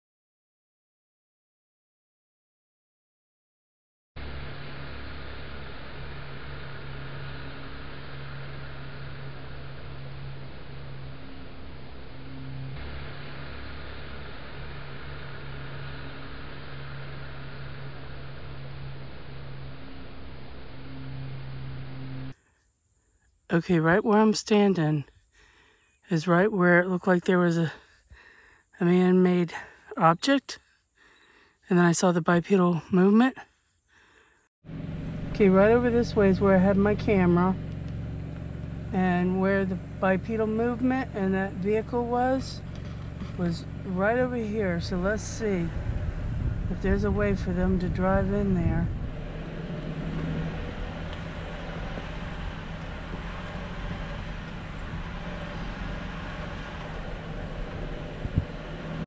Something Strange And Bipedal In The Green Swamp!
I can't explain what is going on out here. My game camera picks up something that looks man made like a truck in the woods that disappears 4 seconds later. There are no roads or tire marks in this area when I tried to debunk it. Then you see bipedal movement. Is it Bigfoot? Is it a UFO? I really don't know. You will see where I zoom into the object and the bipedal movement.